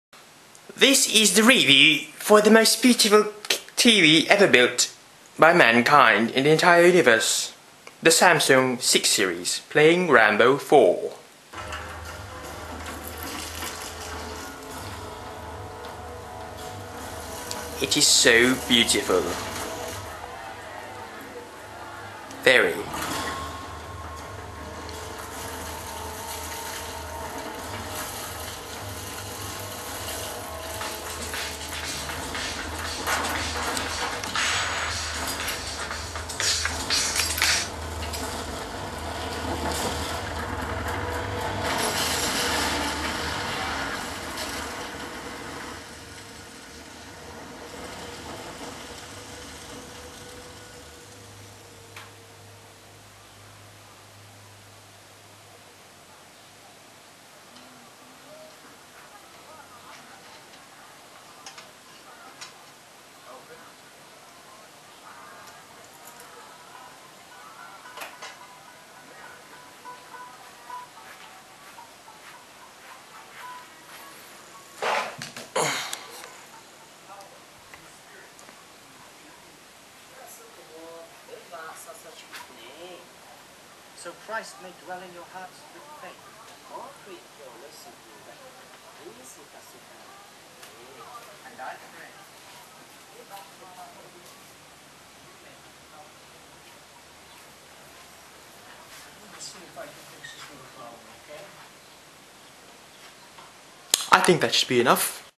New Samsung 6 series 40" LA40A650A1R 1080p
New Samsung 6 series LA40A650A1R playing a normal dvd video. Full HD 1080p 24p, 100Hz Motion Plus, 4ms response time, 50000:1 contrast ratio! "Freaky" and "fake" looking at first due to the fluidity and vibrance of the video played and I haven't even tested it with an HDMI cable nor an HD source!